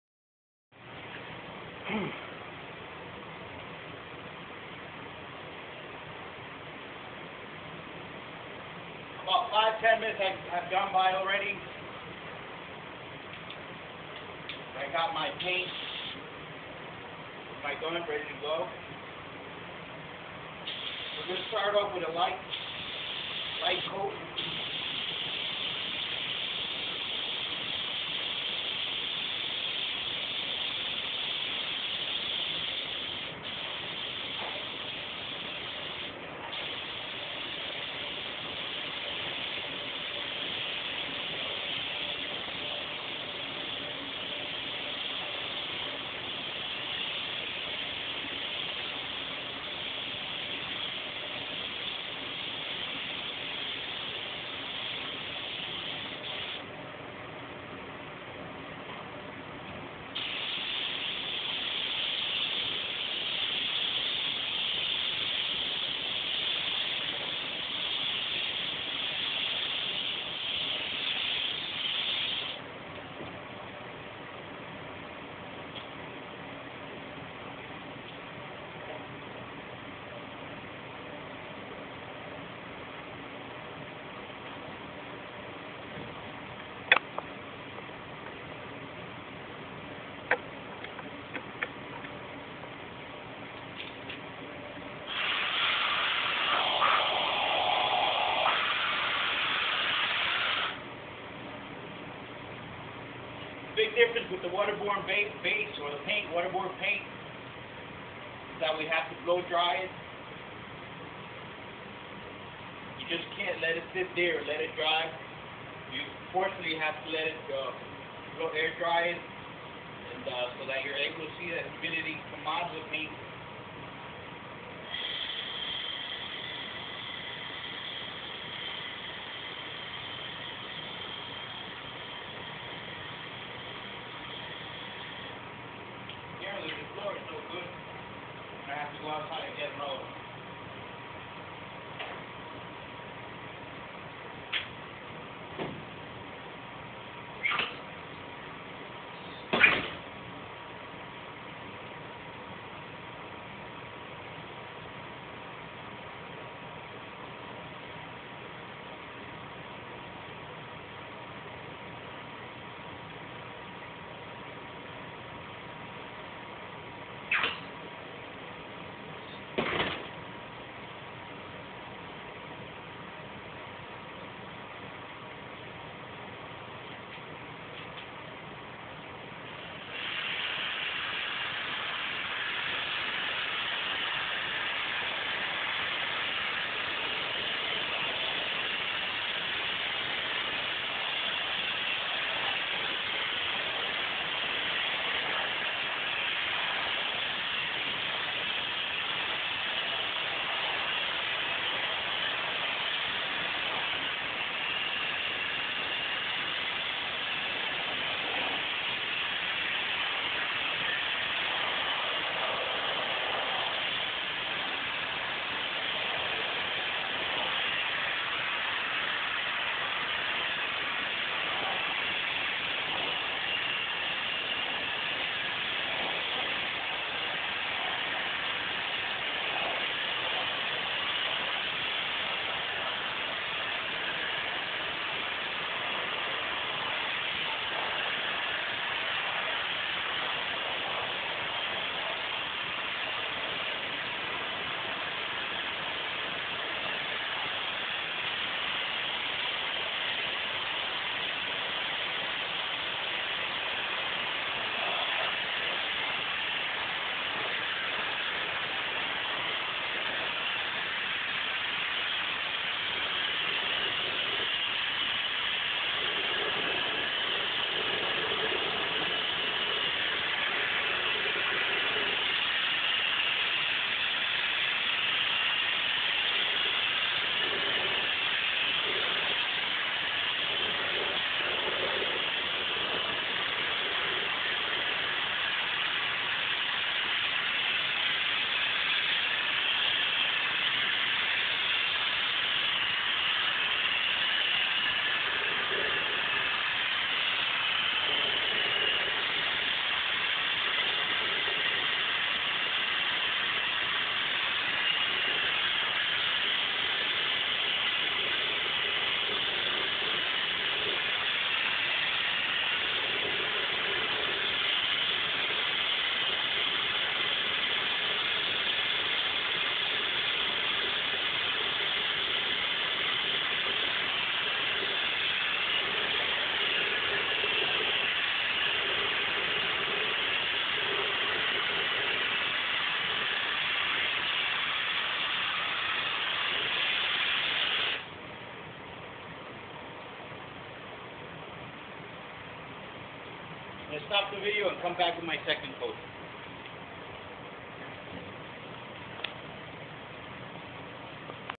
here i am painting a 2008 mini cooper.waterbase
painting a mini cooper.using waterbase paint.